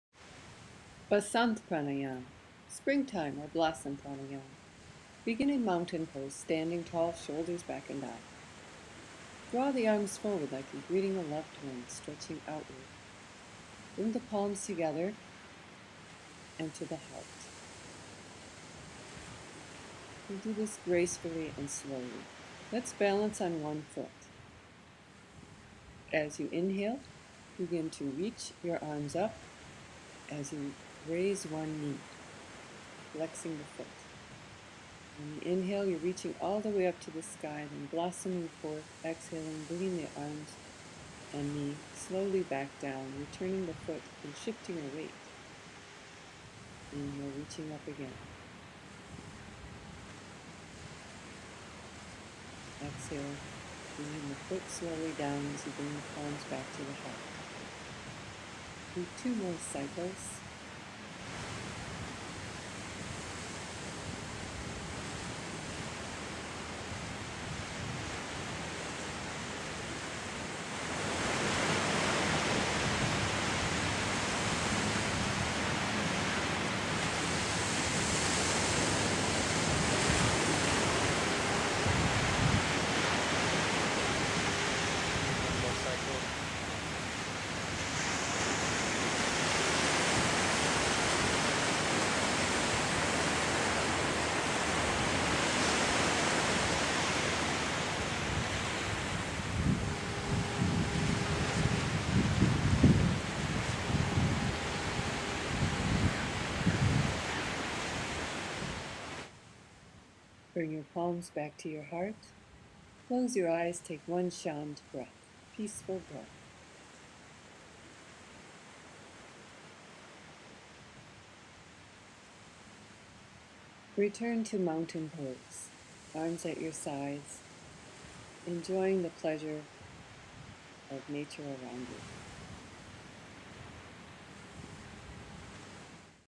Balanced Breathing by RussaYog
This pranayam opens the heart while challenging the mind through balance and coordination. Open yourself to new opportunities while performing blossom pranayam. Enjoy the feel of springtime and new beginnings!